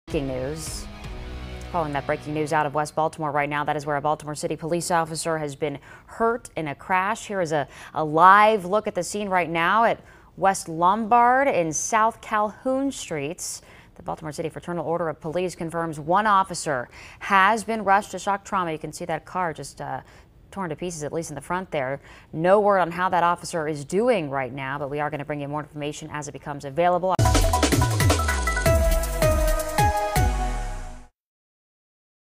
BPD officer injured in on-duty motor vehicle accident
Baltimore City police union confirms an officer was injured in an on-duty motor vehicle accident in Tuesday night in west Baltimore.